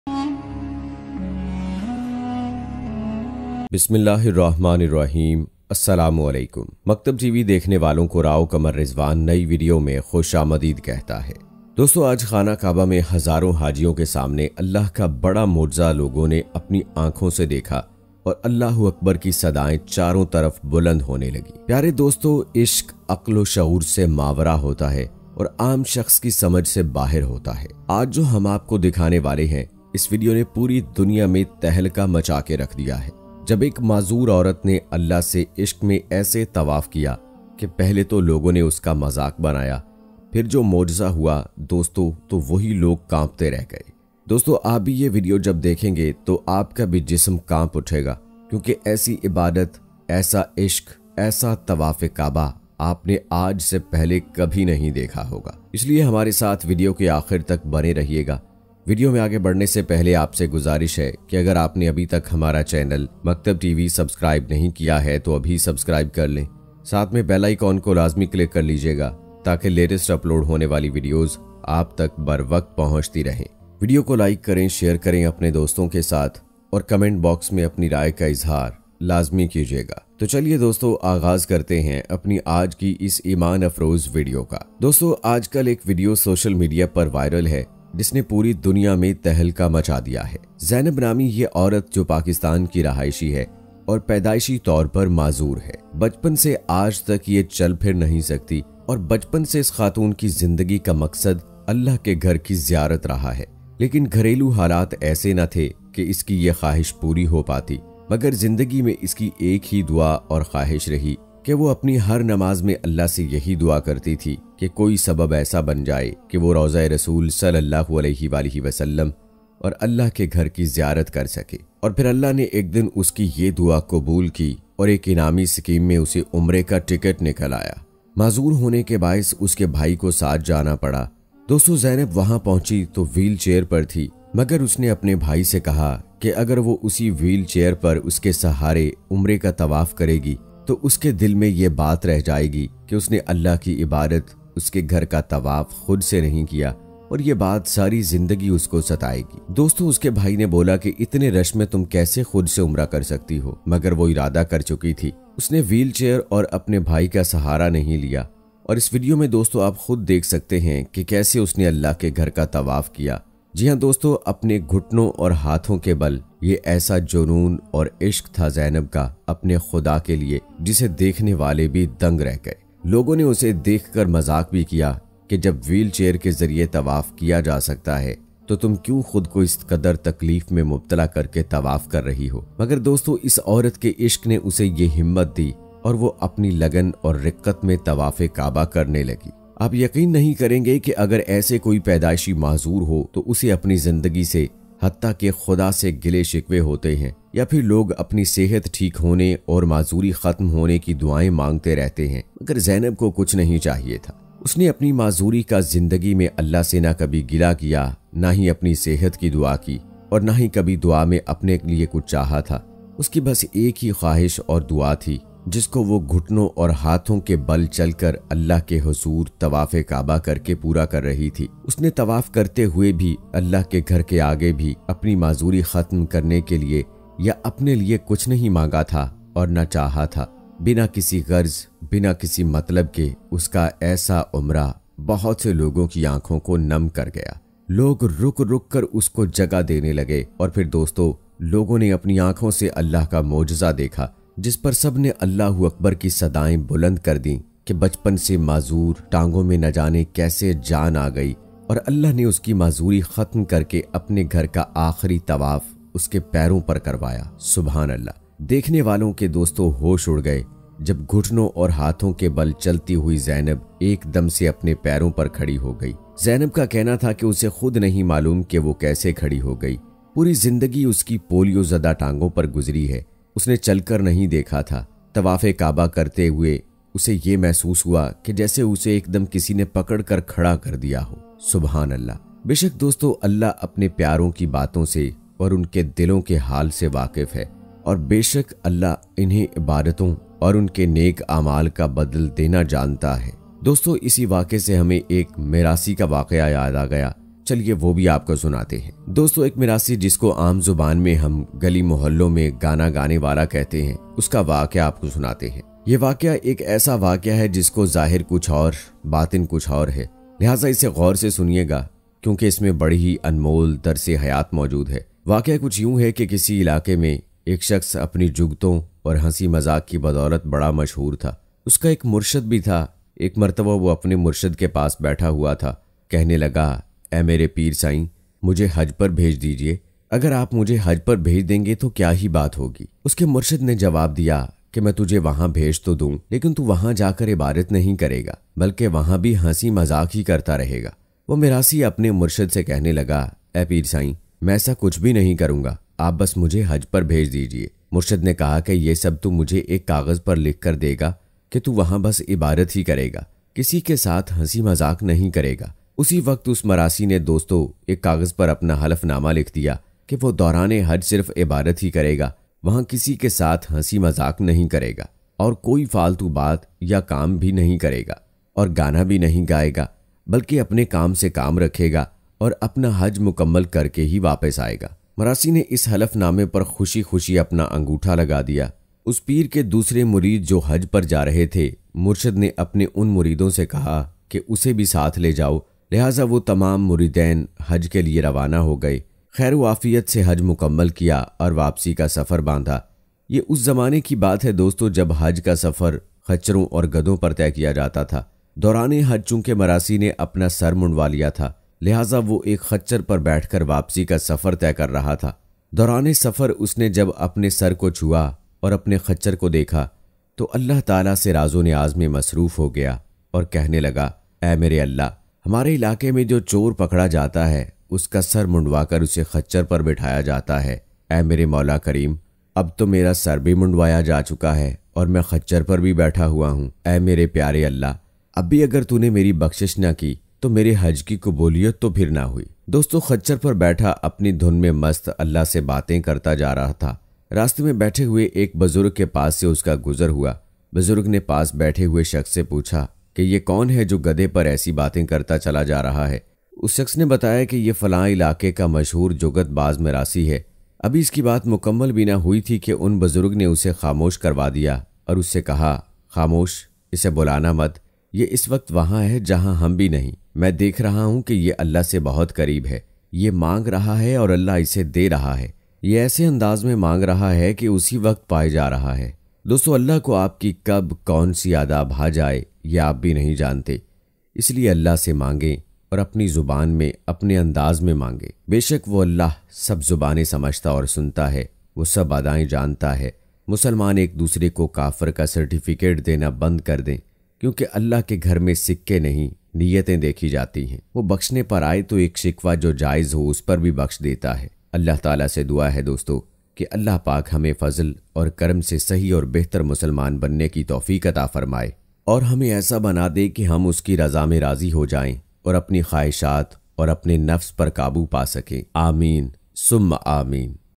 Khana Kaba Main Allah Pak Ka Bhara Mojza Ronuma | خانہ کعبہ میں اللہ کا معجزہ | Maktab TV
دوستو آج کی اس ویڈیو میں آپ کو خانہ کعبہ میں ہونے والے اللہ پاک کے ایک ایسے معجزہ کے بارے میں بتائیں گے کہ جس کو دیکھ کر خانہ کعبہ میں اللہ اکبر کی صدائیں بلند ہوگئی دوستو یہ معجزہ کیا ہے اور کب پیش آیا اس بارے میں مکمل تفصیل جاننے کے لیے اس ویڈیو کو آخر تک لازمی دیکھے اور ویڈیو کو دوستوں کے ساتھ بھی شیئر کریں